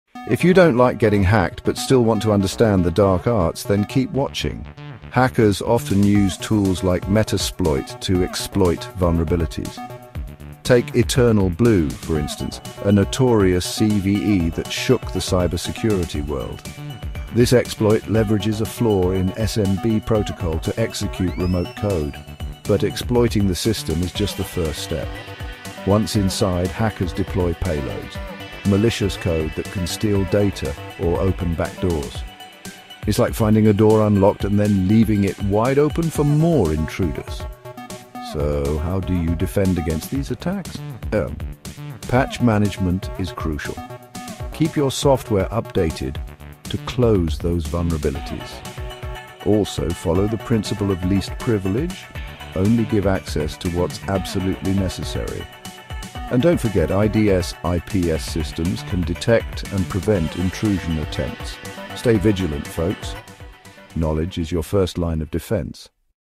How Hackers Infiltrate Systems (Parts 1-5) Part 3: Gaining Access - Exploits andPayloads
In Part 3 of our series, "Unlocking the Hacker's Toolbox:
Exploits & Payloads," we dive deep into how hackers gain access to systems through various exploits and malicious payloads. Discover the power of Metasploit in remote code execution and learn about real-world examples of CVEs, such as the infamous EternalBlue. Understand how a single vulnerability can be the gateway for attackers, and explore defensive strategies like patch management and the principle of least privilege. Equip yourself with knowledge to fortify your systems against cyber threats.